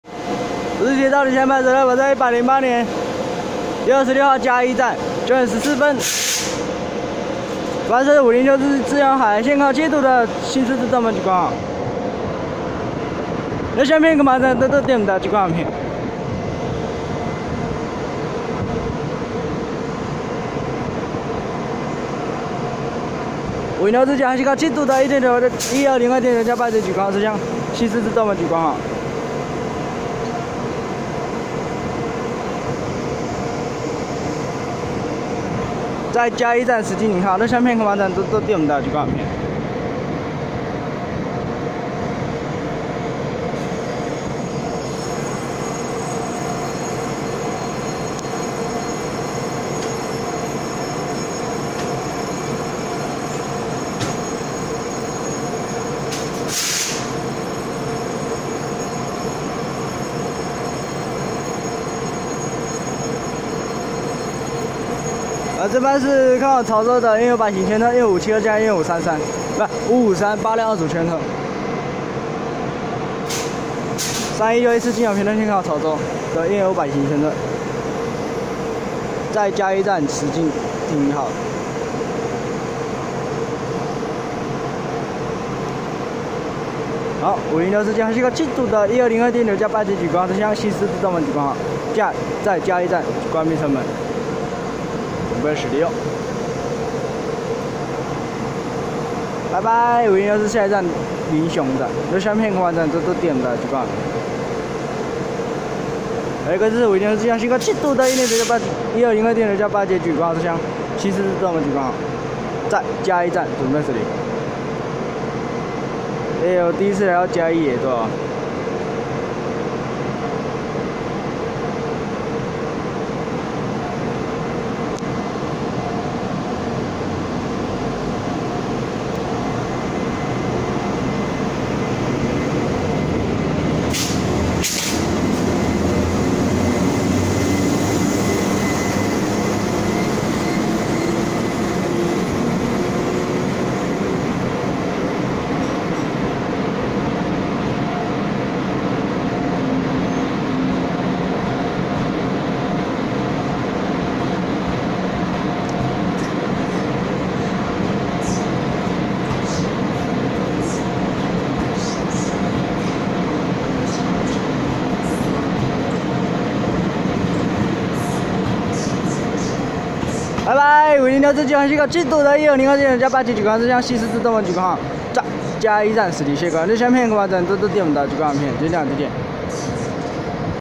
506次經由海線開往七堵的E202電頭+八節莒光號車廂新式自動門莒光號
108/1/26 嘉義站 9:14拍攝的506次經由海線開往七堵的E202電頭+八節莒光號車廂新式自動門莒光號